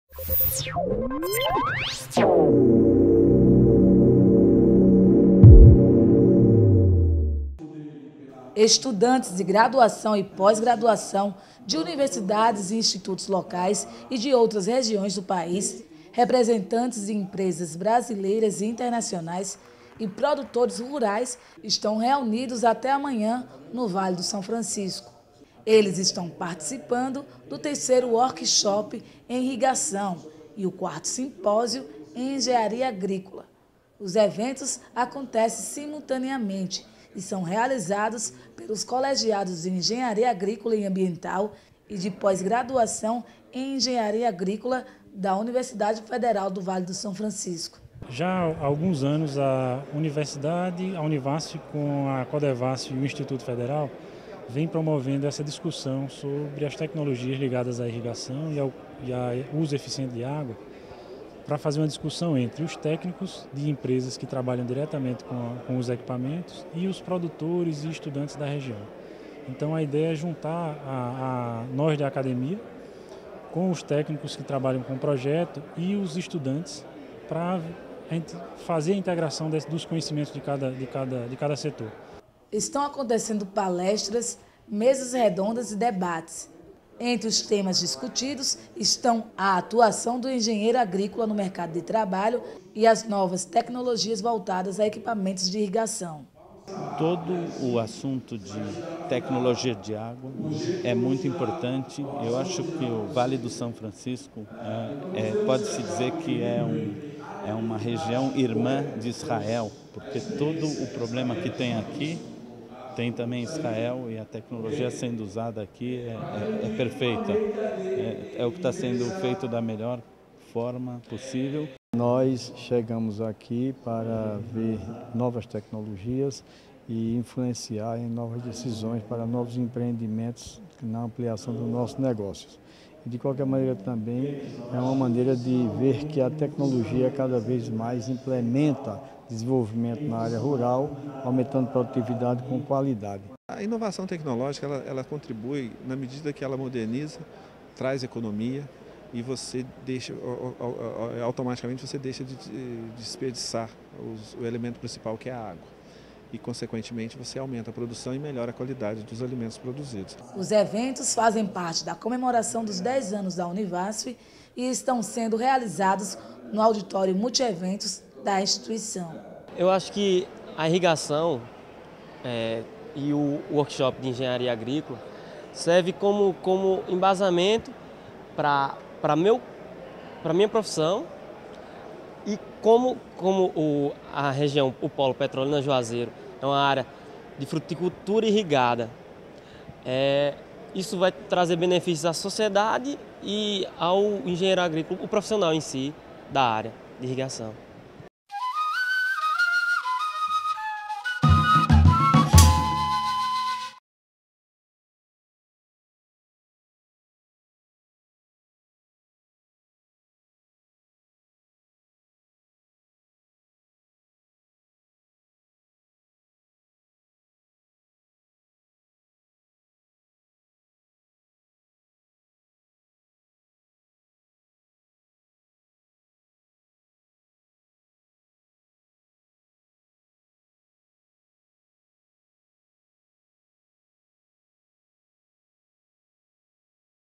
III Workshop de Irrigação
O Vale do São Francisco recebe durante três dias representantes do setor agrícola e da irrigação do Brasil e de fora do país. Eles estão participando de um evento que integra universidades, empresas do setor e produtores rurais.